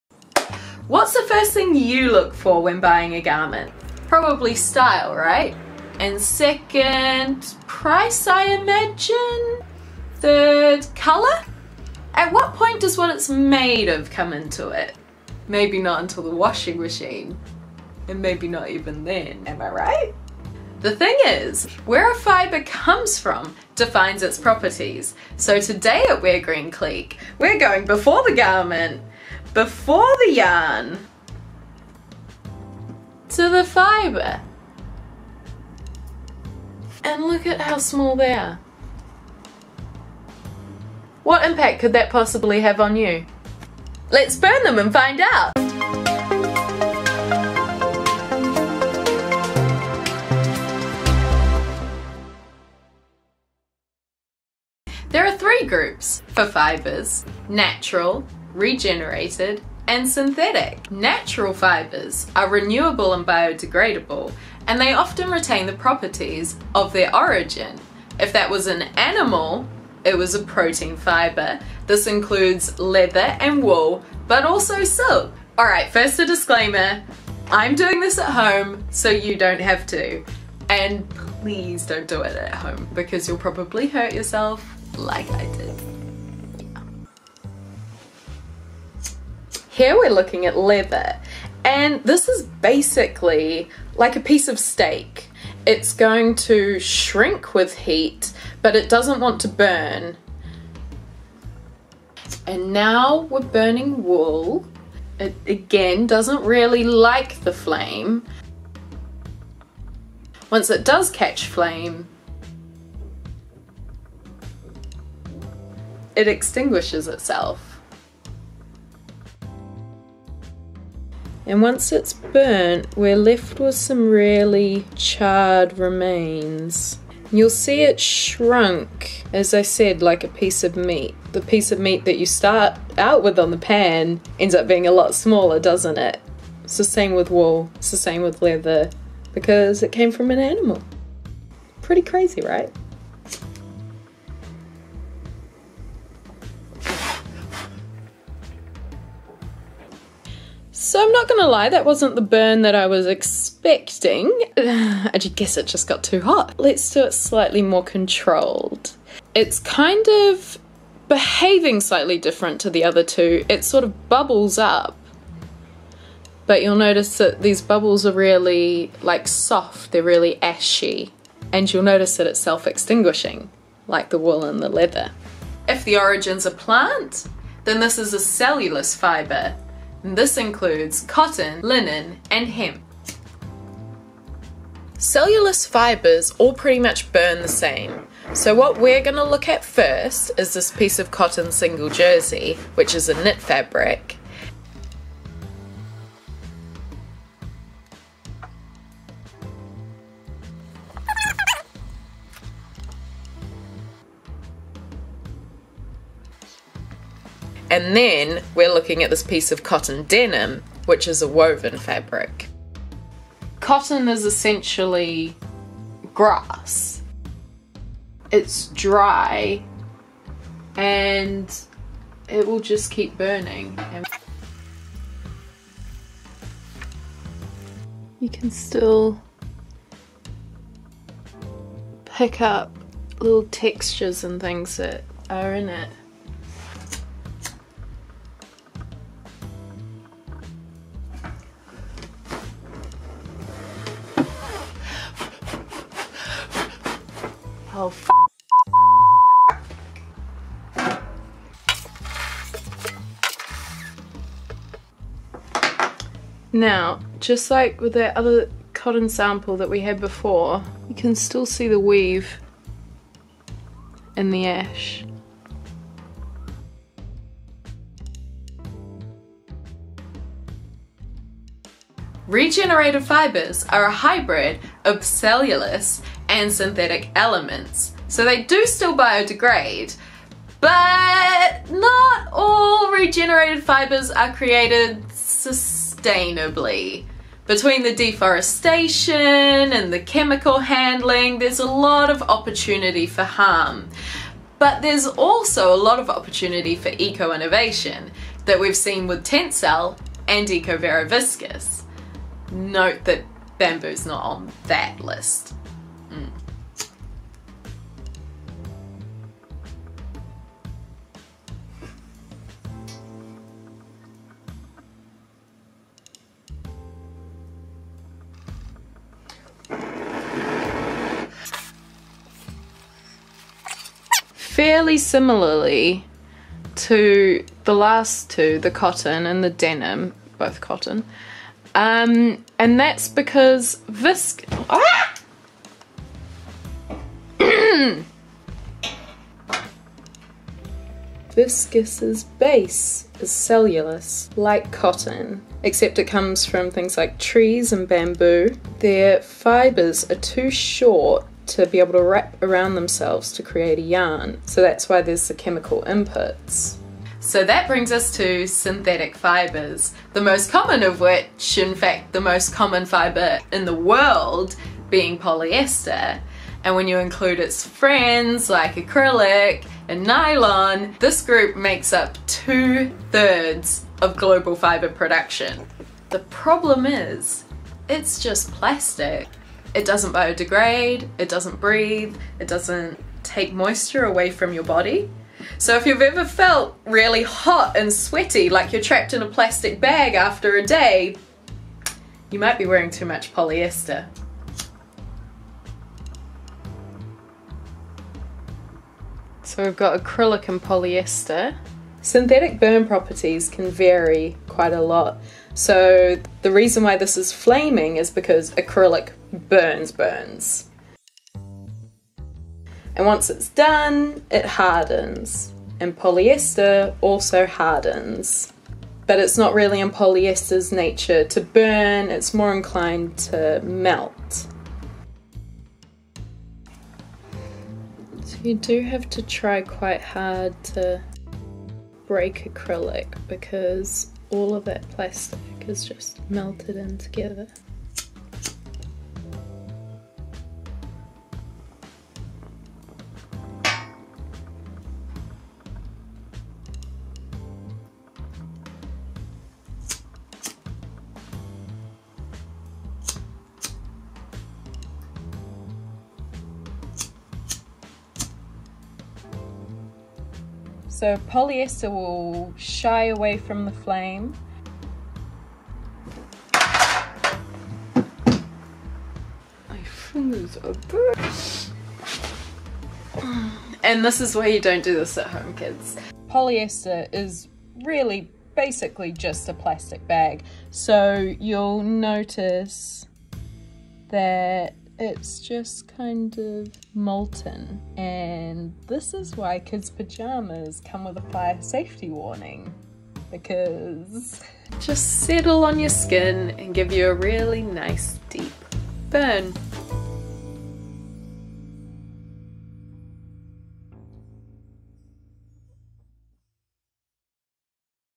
SETTING FIRE TO FIBRES WGC-E3
*DO NOT DO THIS AT HOME* I'm setting fire to fabrics so you don't have to!

What will burning fabrics teach us?
-That the origins of fibres have a huge effect on how they behave...
Which is important for knowing how they can work for you...
And against you.

There'll be some names you know and some you won't, but becoming familiar with fabrics and what they offer will help you to select the best fibres for your wearing comfort and the longevity of your garments

I'll be posting further notes on the properties of these fibres on the WGC website shortly 👀
Join the Clique! to be notified by email!